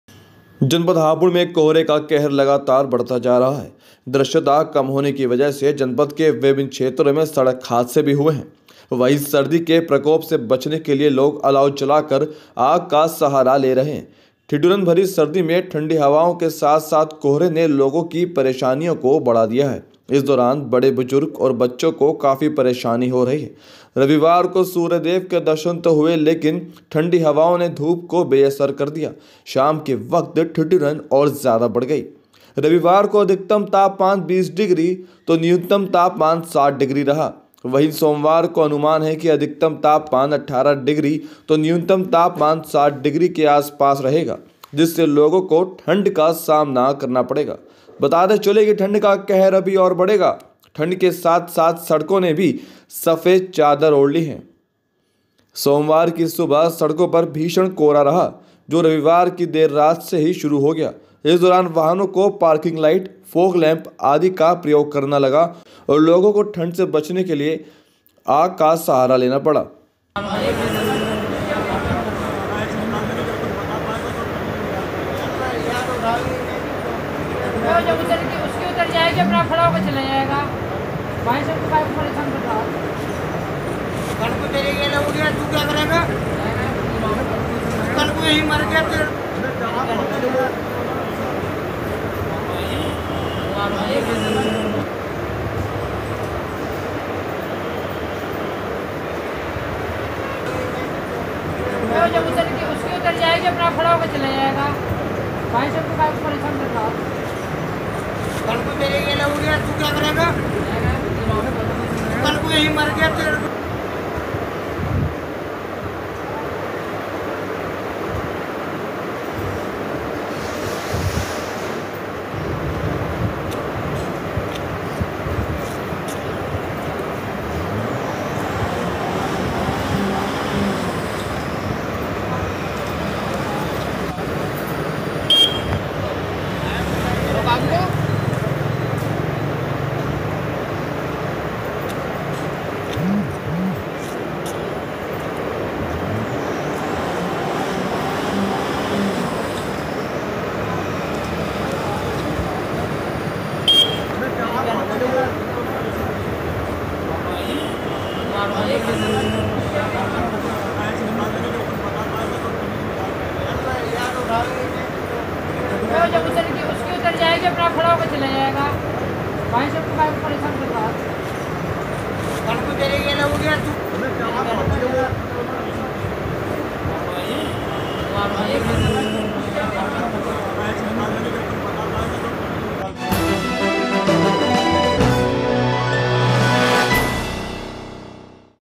सोमवार को भी सड़कों पर छाया भयंकर कोहरा
जनपद हापुड़ में कोहरे का कहर लगातार बढ़ता जा रहा है। दृश्यता कम होने की वजह से जनपद के विभिन्न क्षेत्रों में सड़क हादसे भी हुए हैं। वहीं सर्दी के प्रकोप से बचने के लिए लोग अलाव जलाकर आग का सहारा ले रहे हैं। ठिठुरन भरी सर्दी में ठंडी हवाओं के साथ-साथ कोहरे ने लोगों की परेशानियों को बढ़ा दिया है। इस दौरान बड़े-बुजुर्ग और बच्चों को काफी परेशानी हो रही है। रविवार को सूर्य देव के दर्शन तो हुए लेकिन ठंडी हवाओं ने धूप को बेअसर कर दिया। शाम के वक्त ठिठुरन और ज्यादा बढ़ गई।
रविवार को अधिकतम तापमान 20 डिग्री तो न्यूनतम तापमान 7 डिग्री रहा। वहीं सोमवार को अनुमान है कि अधिकतम तापमान 18 डिग्री तो न्यूनतम तापमान 7 डिग्री के आसपास रहेगा जिससे लोगों को ठण्ड का सामने करना पड़ेगा। बताते चलें कि ठंड का कहर अभी और बढ़ेगा। ठंड के साथ-साथ सड़कों ने भी सफेद चादर ओढ़ ली है। सोमवार की सुबह सड़कों पर भीषण कोहरा रहा जो रविवार की देर रात से ही शुरू हो गया। इस दौरान वाहनों को पार्किंग लाइट, फोग लैंप आदि का प्रयोग करना पड़ा।